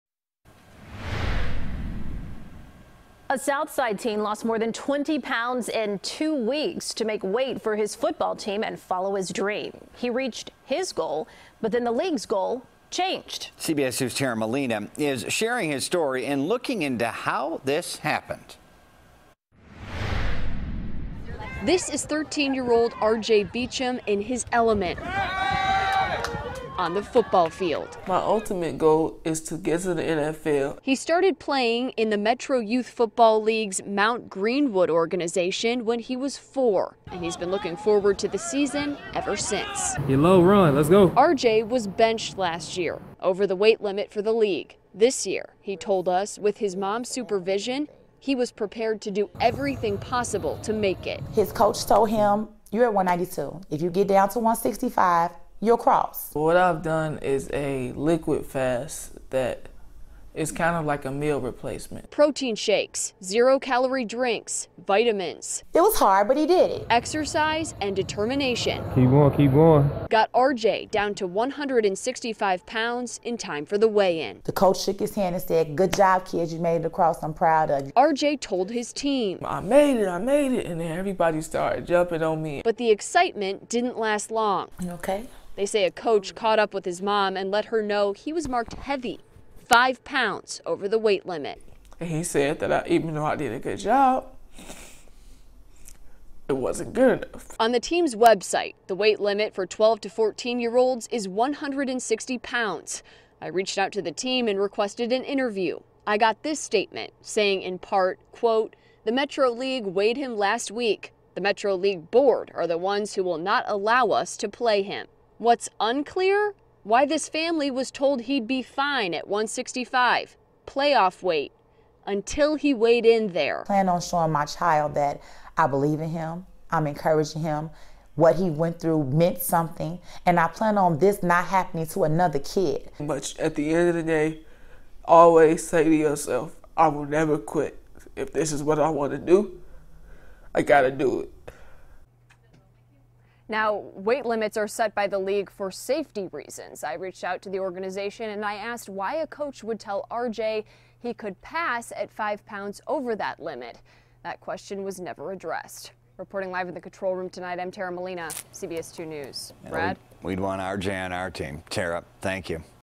Teen Football Player Loses Over 20 Pounds, But It Doesn't Matter
R.J. Beacham went through painstaking efforts to lose weight so he could play for his Mount Greenwood Colts football team. But then the team's weight goals changed. CBS 2's Tara Molina reports.